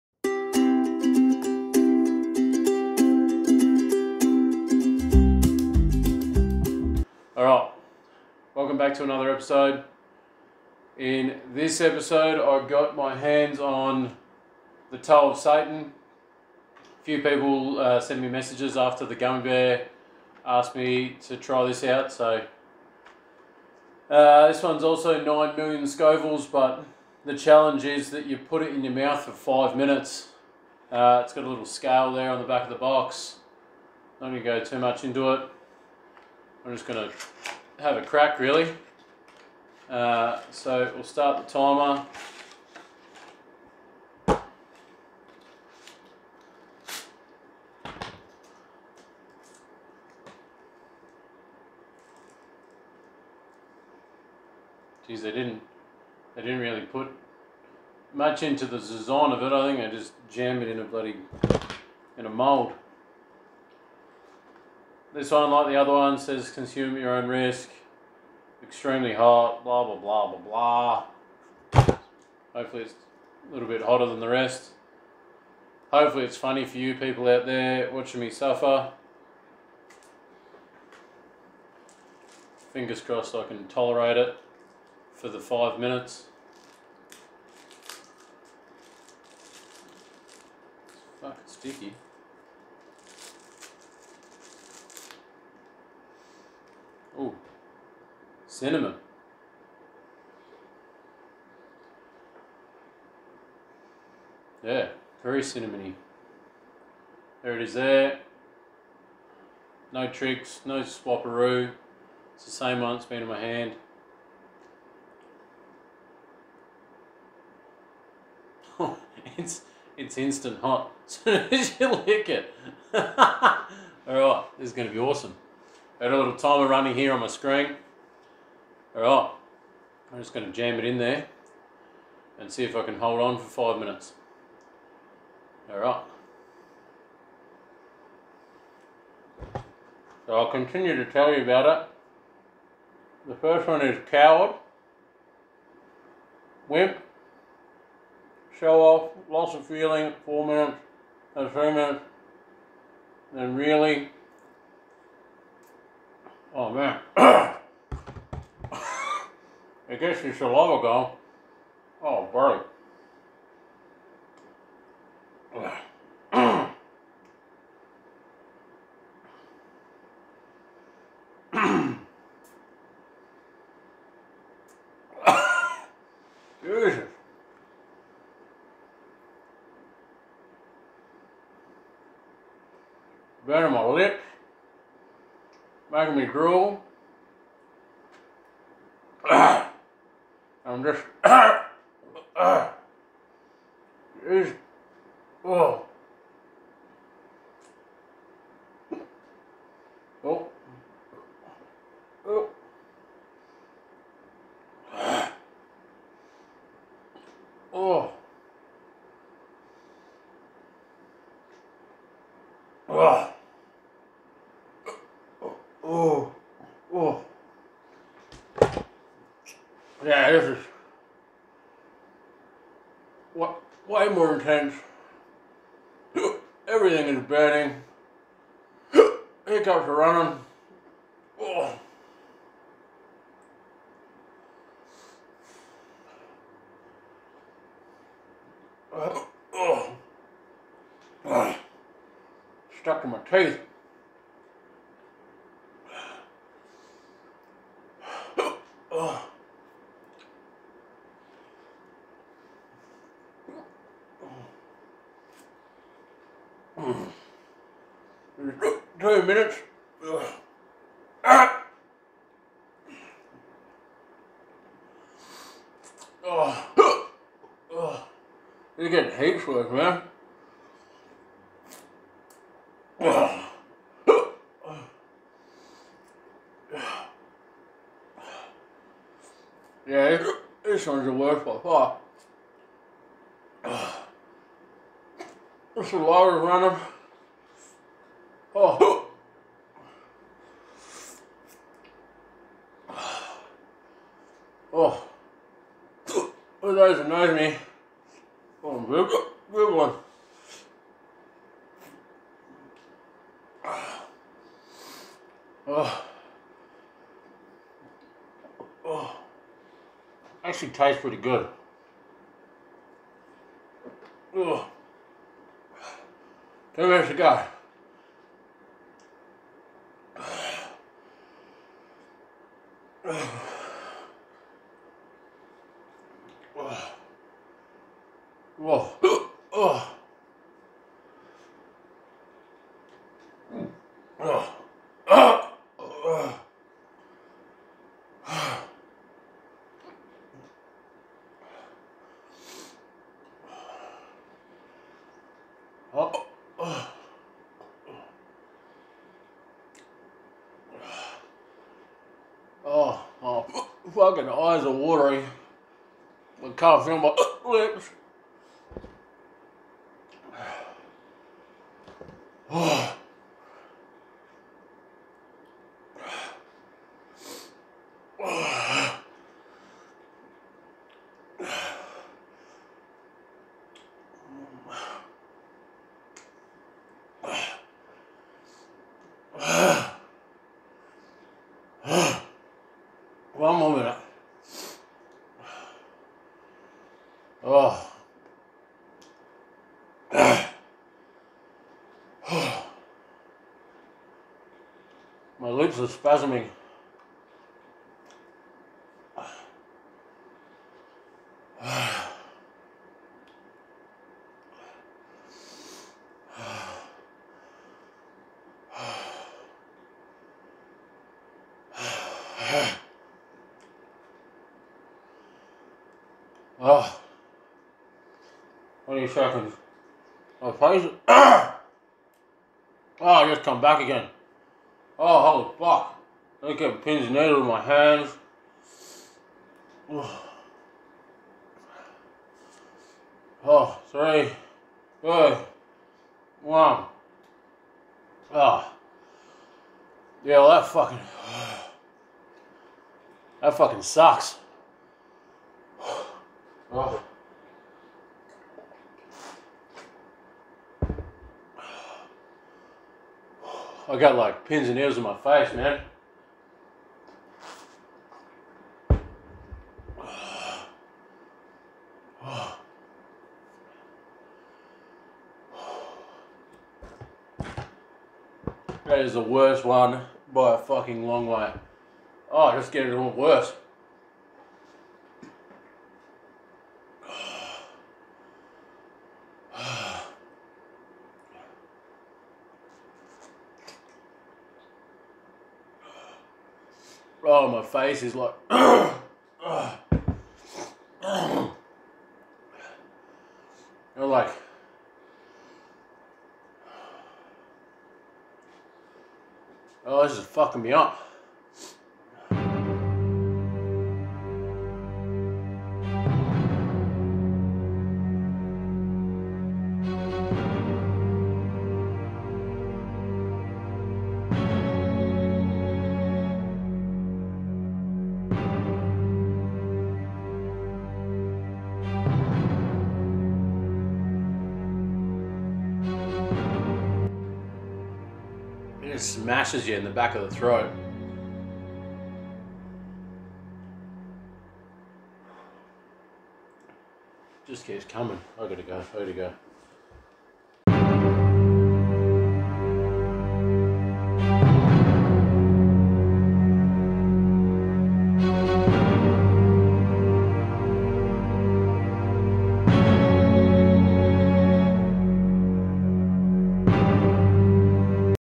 SATANS TOE 9 MILLION SOVILLE( HUGE FAIL ) Cam Classified
The Toe of Satan Lollipop is not a sweet sucker to savour. It’s a candy that tests your mental while it scorches your tongue.

This satanic sucker is made from a special "hellfire" chili extract that has a Scoville heat unit rating of 9 million—that’s 900 times hotter than a jalapeño and hotter than the hottest pepper on the planet! So be careful: this is a food for serious spice gladiators only.

If you’re hell-bent on heat, try the Toe of Satan Challenge: keep the sucker in your mouth for five merciless minutes. It’s guaranteed to turn your mouth into a lake of fire.